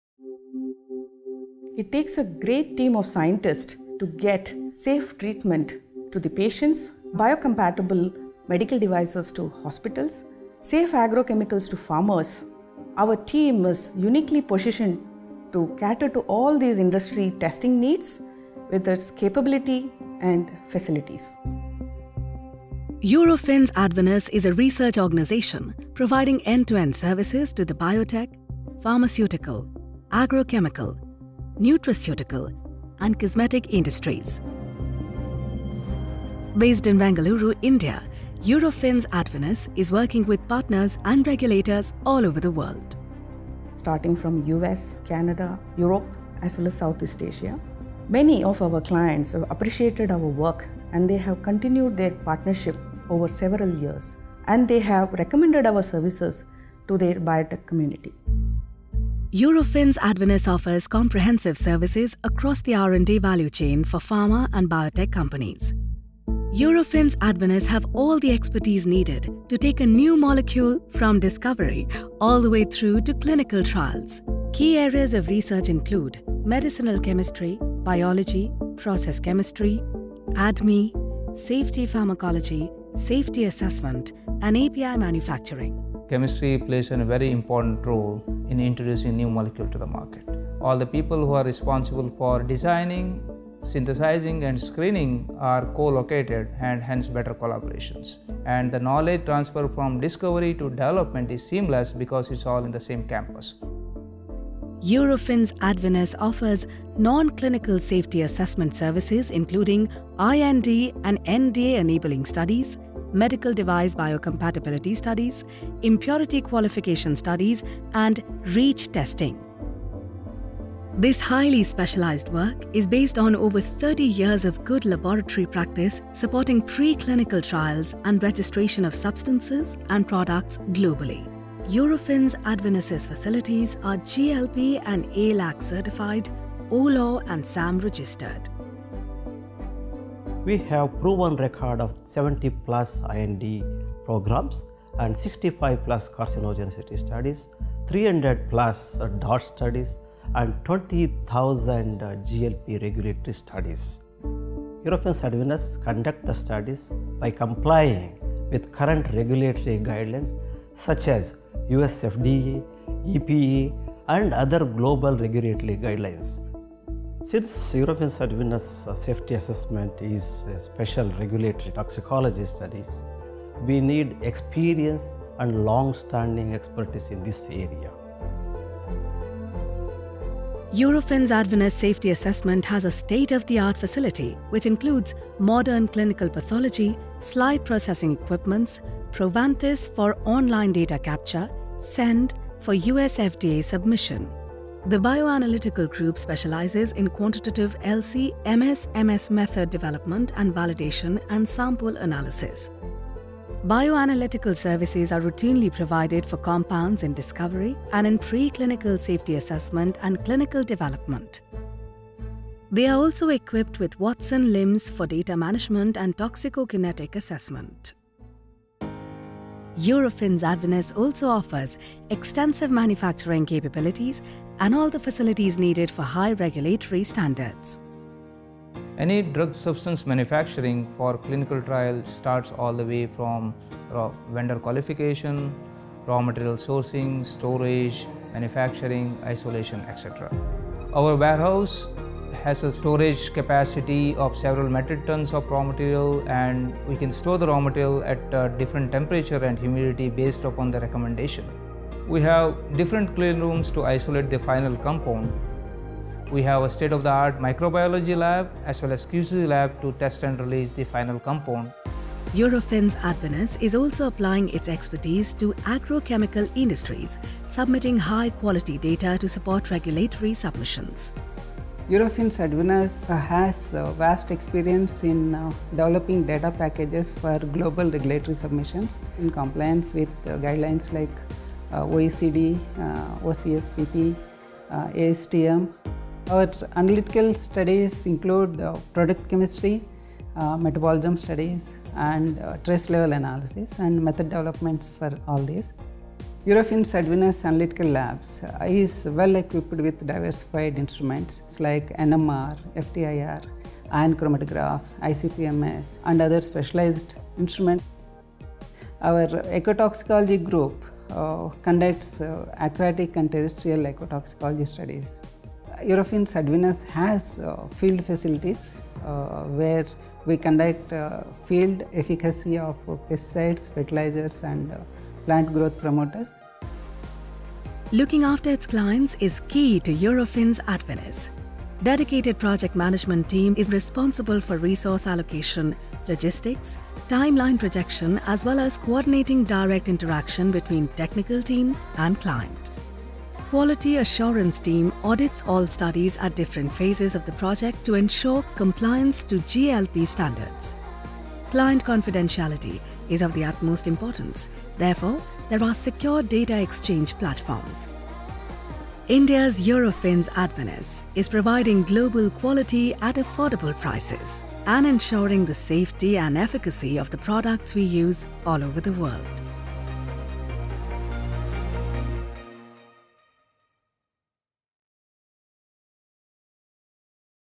Eurofins Advinus - Enabling Research for Better Life
Eurofins Advinus is a research organization providing end-to-end services to the Biotech, Pharmaceutical, Biological, Agrochemical, Nutraceutical and Cosmetic industries. Based in Bengaluru, India, Eurofins Advinus is working with partners and regulators all over the world.

Eurofins Advinus offers comprehensive services across the R&D value chain for Pharma and Biotech companies. Eurofins Advinus have all the expertise needed to take a new molecule from discovery all the way through to clinical trials. Key areas of research include Medicinal chemistry, Biology, Process chemistry, ADME, Safety pharmacology, Toxicology and API Manufacturing.

Eurofins Advinus is providing toxicology services including Exploratory and Definitive GLP Toxicology , Safety pharmacology, Genetic Toxicology, Developmental and Reproductive toxicology, Carcinogenicity, Medical device Biocompatibility testing and impurity qualification studies.

This highly specialized work is based on over 30 years of good laboratory practice supporting preclinical trials and registration of substances and products globally.

India’s Eurofins Advinus is providing global quality at affordable prices, and ensuring the safety and efficacy of the products we use all over the world.